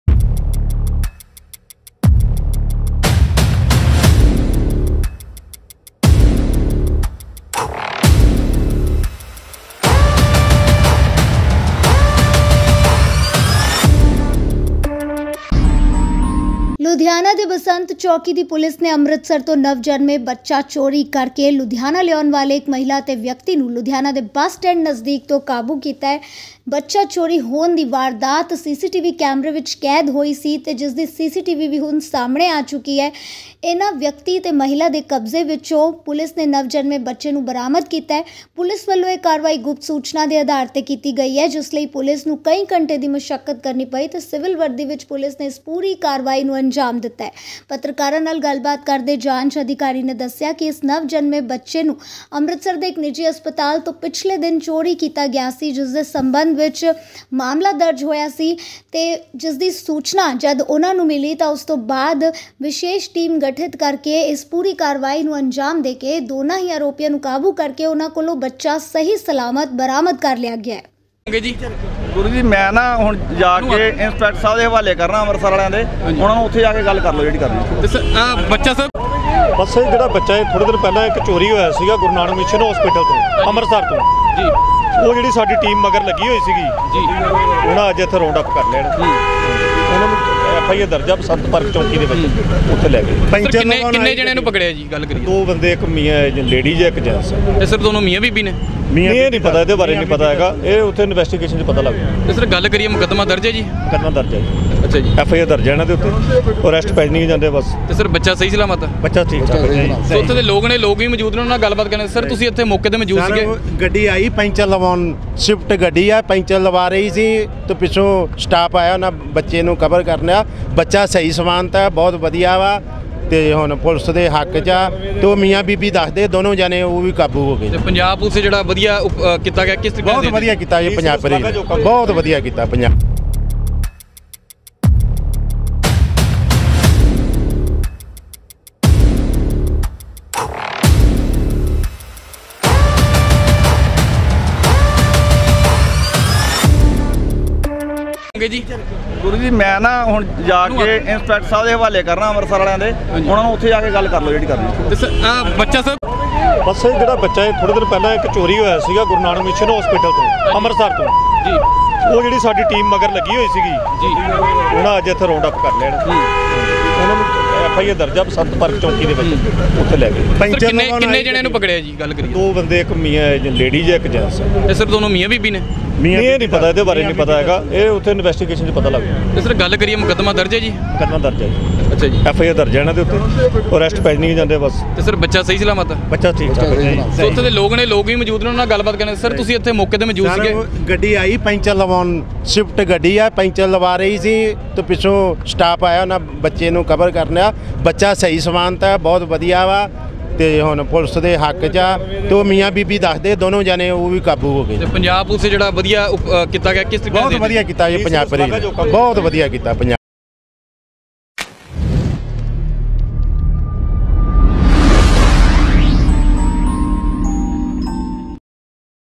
ਬਸ ਸਟੈਂਡ ਤੋਂ ਨਵਜੰਮੇ ਬੱਚੇ ਨੂੰ ਚੋਰੀ ਕਰਕੇ ਲਿਆਉਣ ਵਾਲੇ ਮਹਿਲਾ ਤੇ ਵਿਅਕਤੀ ਨੂੰ ਪੁਲਿਸ ਨੇ ਕੀਤਾ ਕਾਬੂ
ਬੱਚਾ ਵੀ ਹੋਇਆ ਬਰਾਮਦ , ਪੁਲਿਸ ਨੇ ਆਰੋਪੀਆਂ ਤੋਂ ਬੱਚਾ ਵੀ ਕੀਤਾ ਬਰਾਮਦ